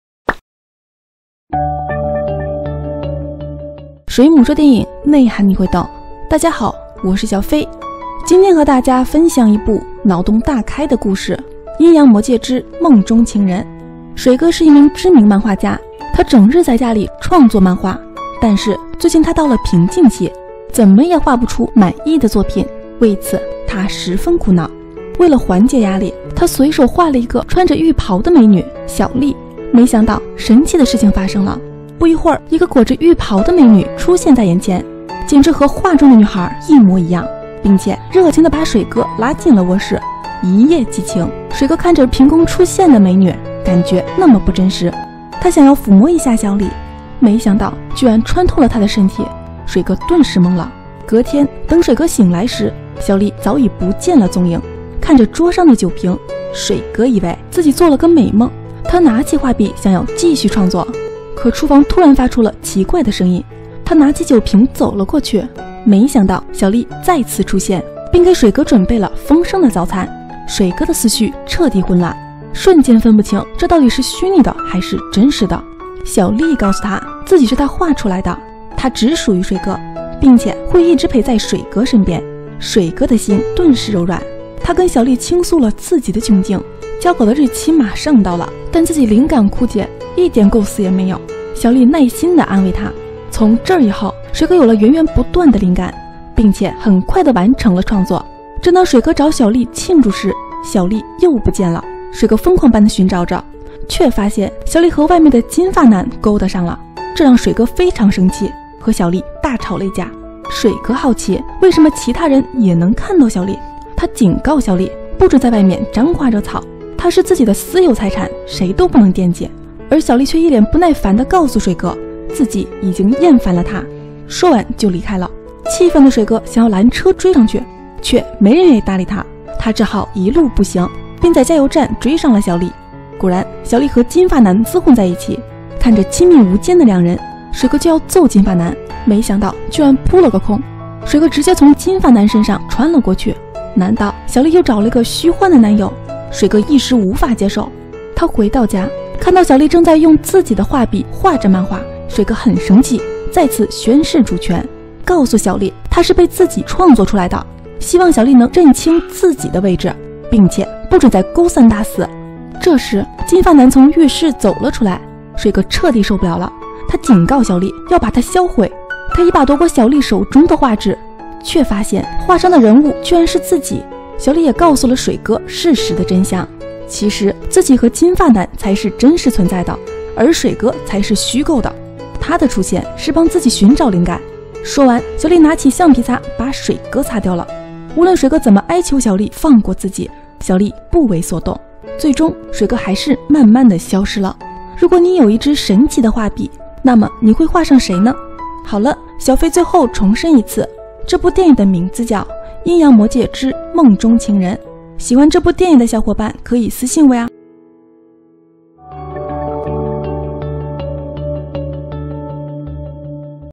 畫家偶然發現一支神筆，畫什麼都成真，終於他畫活了一個婀娜美女
畫家偶然發現一支神筆，畫什麼都成真，於是他畫活了一個美女